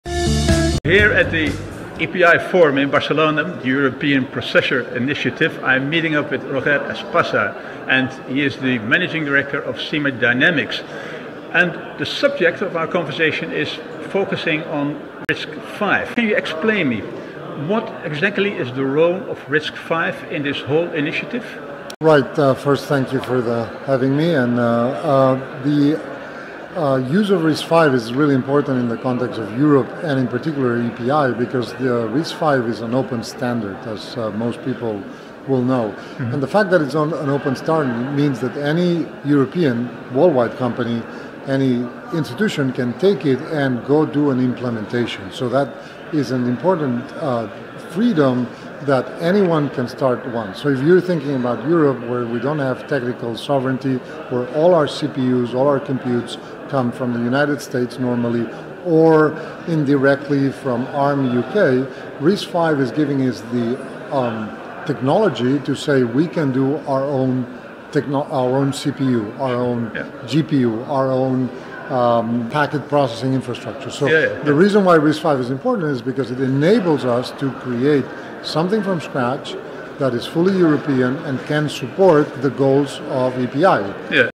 How RISC-V is Powering Europe’s Tech Independence | Insights from the EPI Forum in Barcelona
Join us at the EPI Forum in Barcelona, where we delve into the European Processor Initiative (EPI) and explore the groundbreaking role of RISC-V in Europe's tech landscape. In this video, we sit down with Roger Espasa, Managing Director of Semidynamics, to discuss how RISC-V is empowering Europe with a new level of technological independence and sovereignty.

RISC-V, an open standard, provides unprecedented freedom for European companies and institutions to develop and implement their own processors, GPUs, and packet processing infrastructure. This open-source architecture is crucial for Europe, offering a unique opportunity to establish a fully European-driven technology ecosystem. Unlike traditional CPUs, largely controlled by non-European entities, RISC-V allows Europe to innovate without relying on external technologies.

Tune in to learn how RISC-V is a pivotal part of the EPI’s mission to foster Europe’s capability in designing and controlling its own computational technologies—an essential step toward European sovereignty in tech.